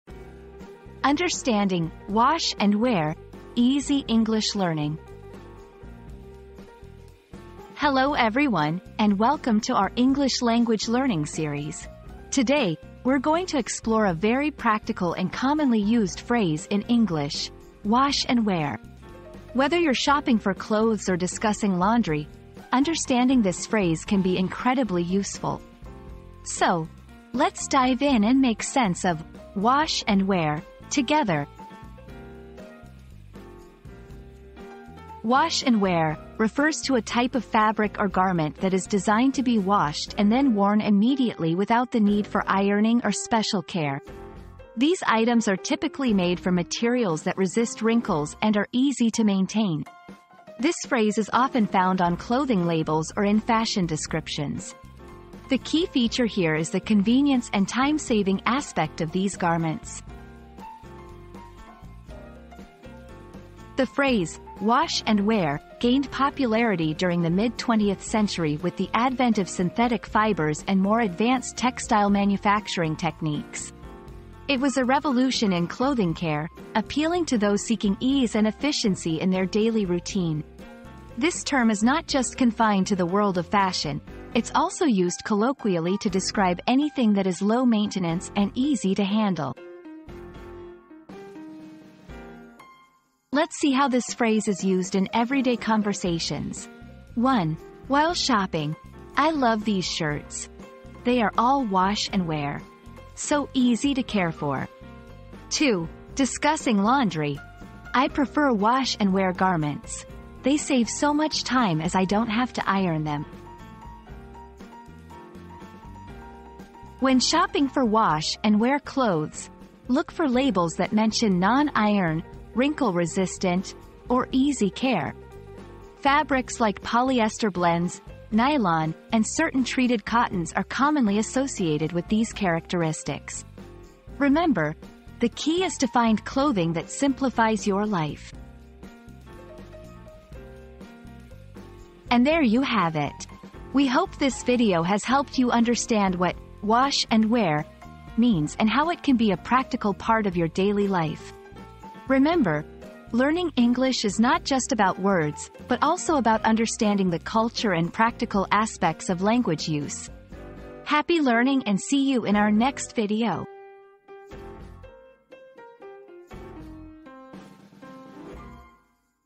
Understanding "Wash and Wear" - Easy English Learning
00:00 • Introduction - Understanding "Wash and Wear" - Easy English Learning
00:34 • What Does "Wash and Wear" Mean?
01:03 • Origins and Usage of the Phrase
01:32 • Examples in Everyday Conversations
01:56 • Practical Tips When Buying "Wash and Wear" Clothes